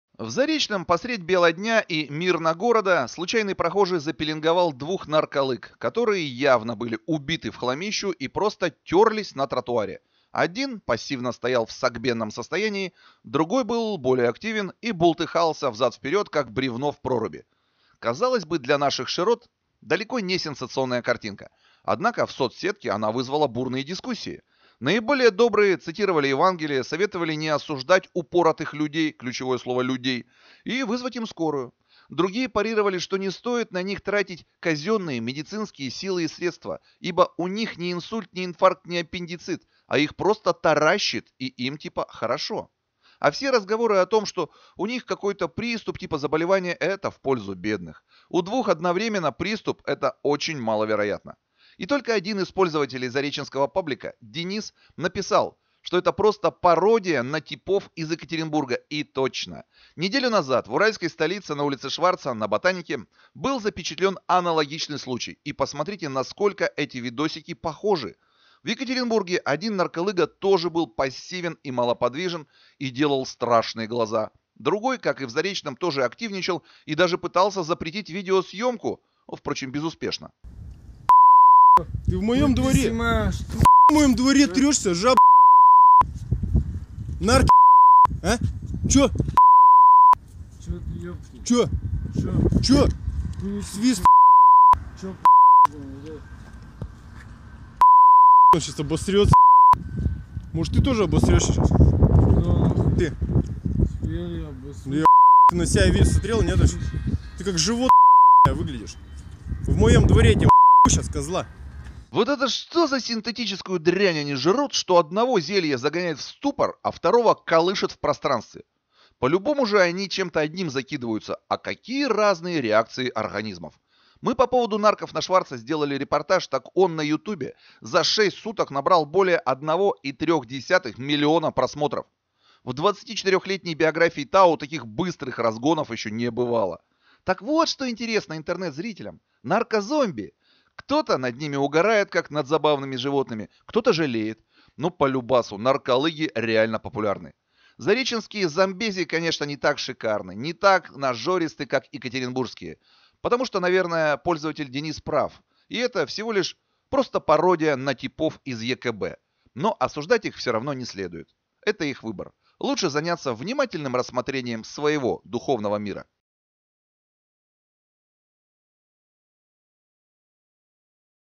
И снова наркозомби! Часть 2. Real video
Картинка один в один: один в ступоре, другой колышется